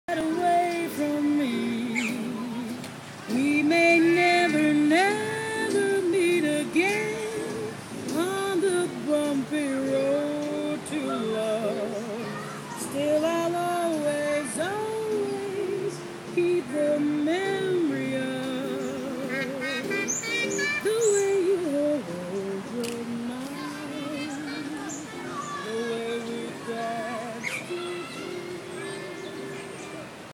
Jake's birthday spot
This is where Jake celebrated his first birthday